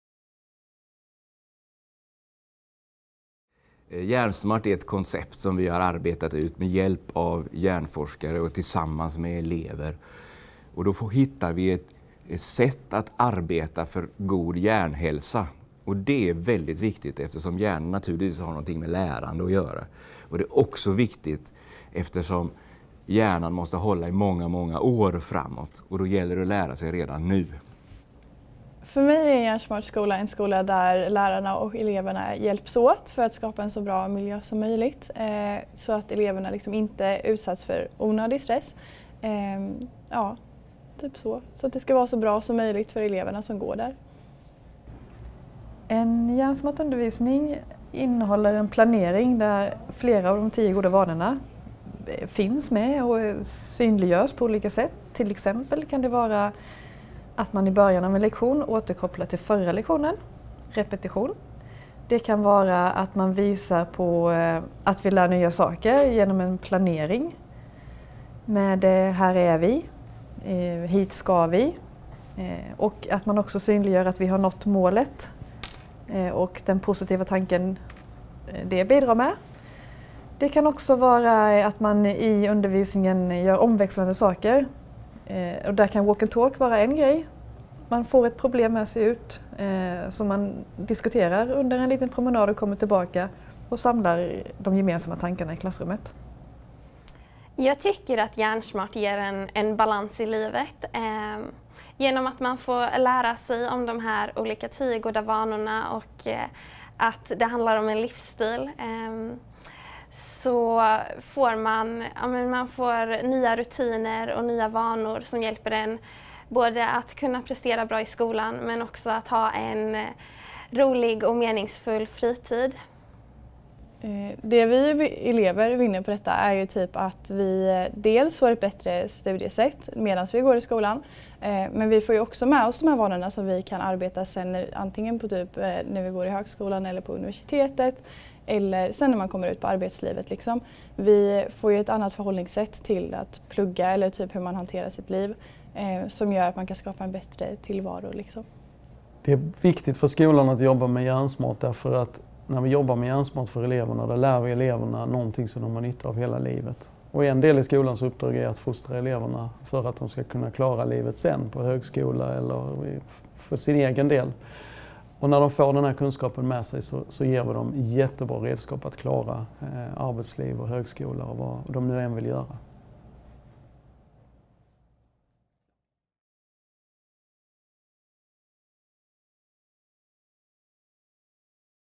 Hjärnsmart i Halmstads kommun
Hur påverkar en god hjärnhälsa dig som individ och samhället i stort? Eleverna på Kattegattgymnasiet använder Hjärnsmart för att öka sina kunskaper om hjärnan samt att skapa en förståelse för att hjärnan är ett fantastiskt verktyg som är formbart och utvecklingsbenäget genom hela livet! För pedagogen innebär Hjärnsmart nya möjligheter till lärandesituationer med en vetenskaplig grund och en verkstad för kreativ pedagogik.

ENGLISH: Brain-Smart - 10 Good Habits in school

How does good brain health affect you as an individual and our society as a whole? The students at Kattegattgymnasiet use the concept Brain-Smart to increase their knowledge of the brain and to create an understanding that the brain is a fantastic tool; adaptive, formable and eager to develop throughout life! For the teacher, Brain-Smart presents new opportunities for learning situations with a scientific basis and a great workshop for creative education.

10 Good Habits:
Food & nutrition
Physical training
Positive thinking
Handling stress
Learn new things
Repetition
Variety & challenges
Make your own decisions
Friends & relationships
Sleep